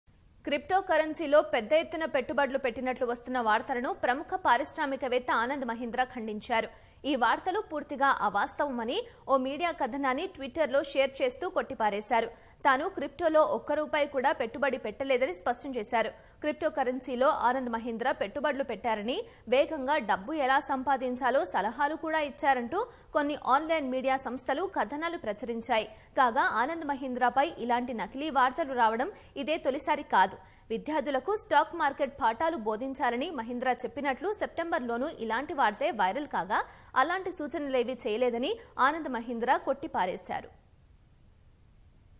క్రిప్టోకరెన్సీలో పెట్టుబడి పెట్టినట్లు వార్తలను ఖండించిన ఆనంద్ మహీంద్రా | Anand Mahindra Denied
క్రిప్టో కరెన్సీలో పెద్దఎత్తున పెట్టుబడులు పెట్టినట్లు వస్తున్న వార్తలను ప్రముఖ పారిశ్రామికవేత్త ఆనంద్ మహీంద్రా ఖండించారు. ఈ వార్తలు పూర్తిగా అవాస్తవమని ఓ మీడియా కథనాన్ని ట్విట్టర్ లో షేర్  చేస్తూ కొట్టిపారేశారు. తాను క్రిప్టోల్లో ఒక్క రూపాయి కూడా పెట్టుబడి పెట్టలేదని స్పష్టం చేశారు. క్రిప్టో కరెన్సీలో ఆనంద్  మహీంద్రా పెట్టుబడులు పెట్టారని.. వేగంగా డబ్బు ఎలా సంపాదించాలో సలహాలు కూడా ఇచ్చారంటూ కొన్ని ఆన్ లైన్  మీడియా సంస్థలు కథనాలు ప్రచురించాయి. కాగా.. ఆనంద్  మహీంద్రాపై ఇలాంటి నకిలీ వార్తలు రావడం ఇదే తొలిసారి కాదు. విద్యార్థులకు స్టాక్  మార్కెట్  పాఠాలు బోధించాలని మహీంద్రా చెప్పినట్లు సెప్టెంబరులోనూ  ఇలాంటి వార్తే వైరల్  కాగా... అలాంటి  సూచనలేవీ చేయలేదని ఆనంద్  మహీంద్రా కొట్టిపారేశారు.